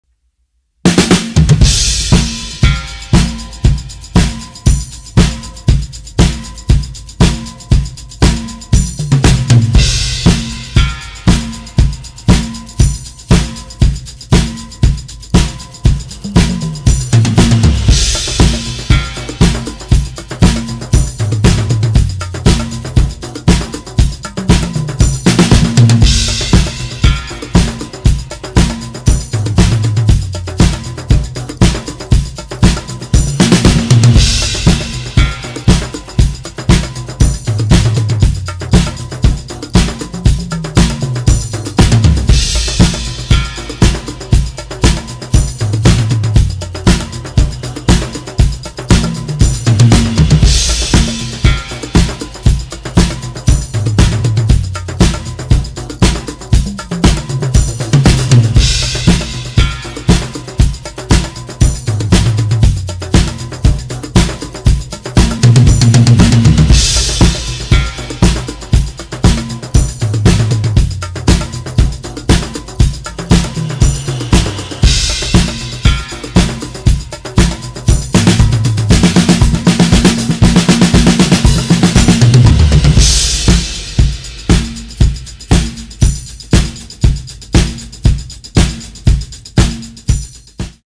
Brendan Buckley: drum/percussion recording session (Henry Dennis) 12/31/20
here’s a drum/percussion breakdown from yesterday’s recording session.
(all live sounds... no loops or samples)
with @henrydennisandthefumesofmars 🎶